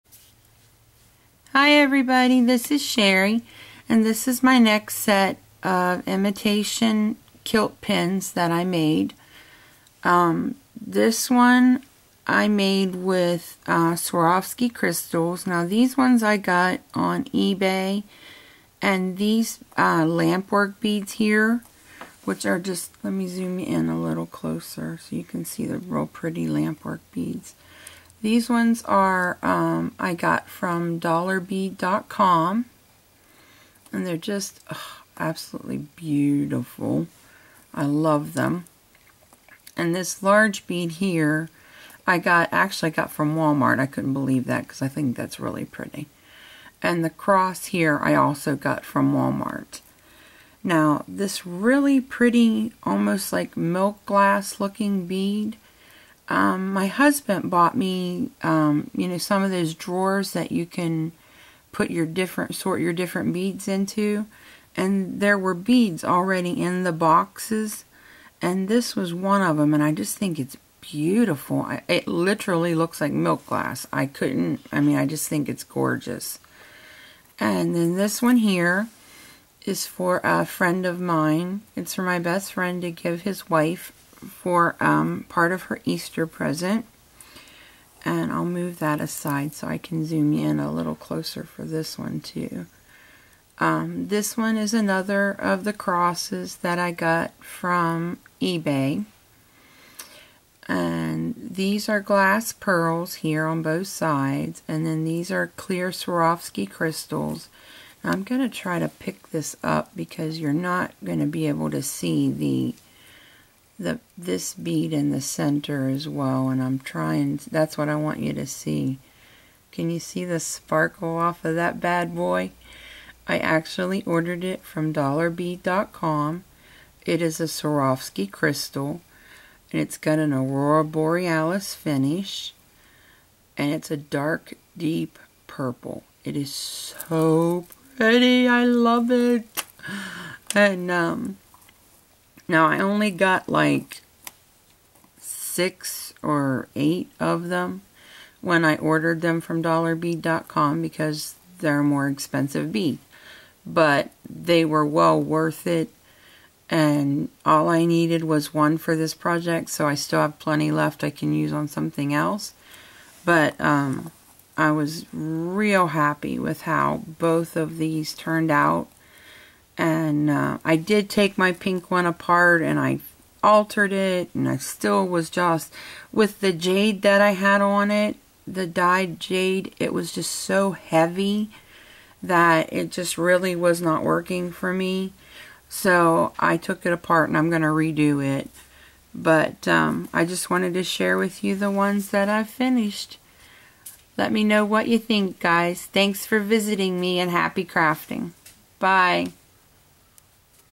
Imitation Kilt Pins
These are so fun to make, so I made 2 more. I'm already planning the design and colors for my next ones. They are a great way to close a decorative bag you have filled for a RAK or swap and also look great on a jacket, blouse or purse as well. I have worn my blue one to church and was just astounded by the compliments I was given. It really made me feel good to know that others thought they were pretty as well.